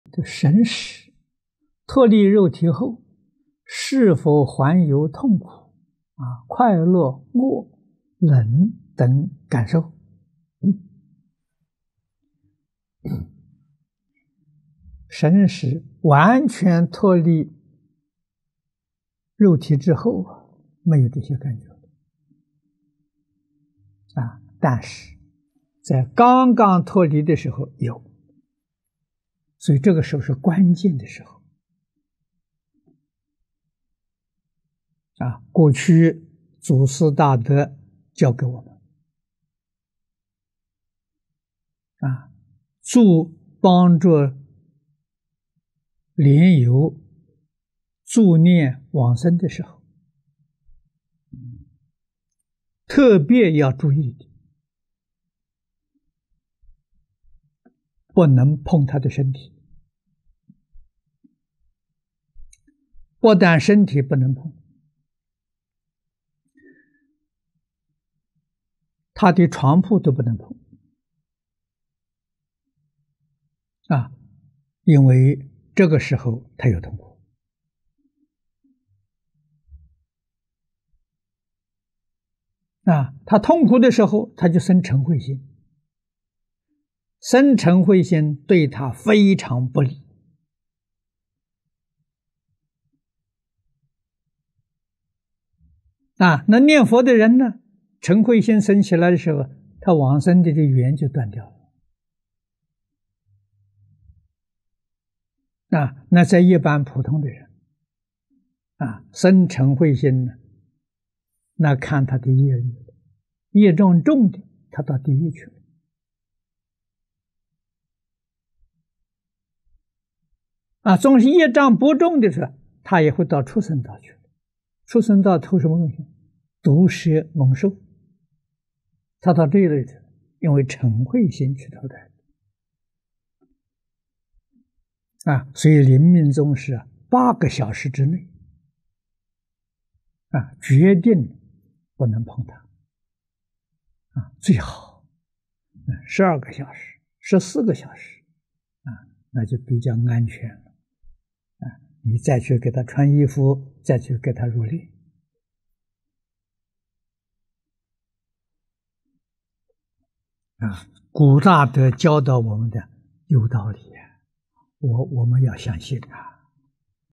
淨空老法師｜學佛答問｜神識脫離身體後，是否還有感知？-第1058集
🌿問：他說神識脫離肉體後，是否還有痛苦、快樂、餓、冷等感受？

🌿答：神識完全脫離肉體之後，沒有這些感覺；但是在剛剛脫離的時候有，所以這個時候是關鍵的時候。

過去祖師大德教給我們，幫著蓮友助念往生的時候，特別要注意的，不能碰他的身體，不但身體不能碰，他的床鋪都不能碰。因為這個時候他有痛苦，他痛苦的時候他就生瞋恚心，生瞋恚心對他非常不利。念佛的人瞋恚心生起來的時候，他往生的緣就斷掉。

在一般普通的人生瞋恚心，那看他的業力，業障重的他到地獄去；縱使業障不重的，他也會到畜生道去。畜生道投什麼東西？毒蛇猛獸，他到這一類去，因為瞋恚心去投胎。

所以臨命終時八個小時之內決定不能碰他，最好十二個小時、十四個小時那就比較安全，你再去給他穿衣服，再去給他入殮。古大德教導我們的有道理，我們要相信！

2009/1/23